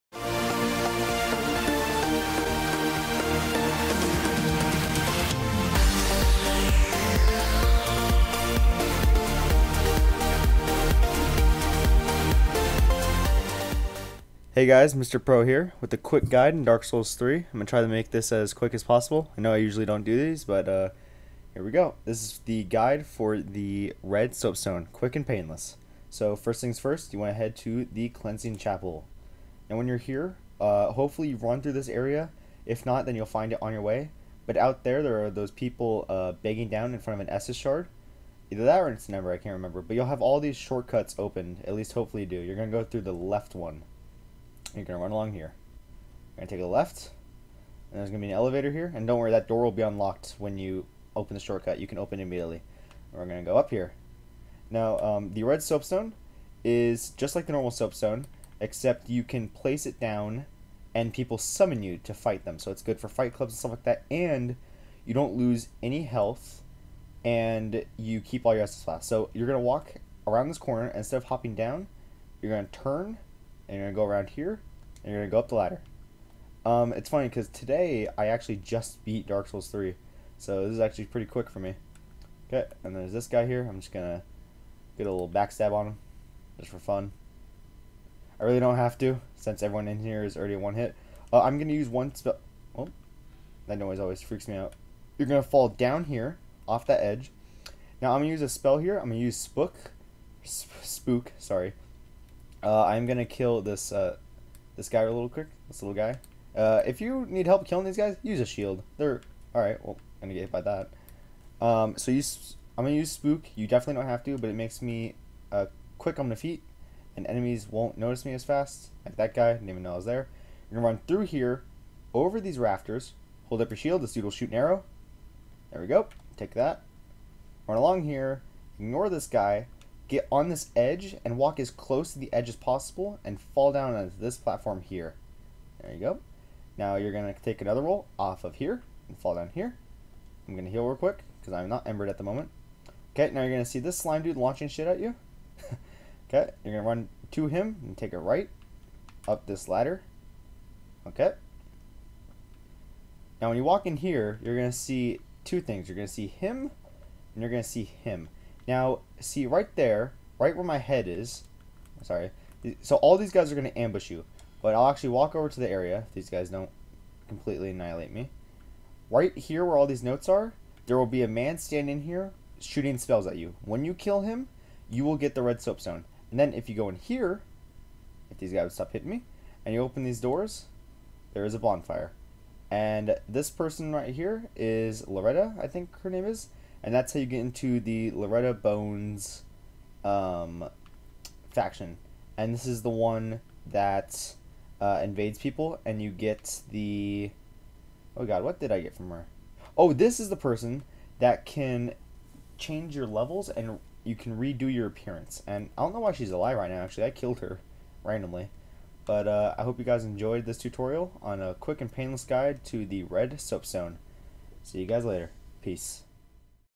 Red Soapstone Location Guide {Dark Souls 3}
Quick guide to red soapstone and Loretta's Covenant. Sorry for lack of game audio. Game recorder did a reset on me and it missed the audio setting.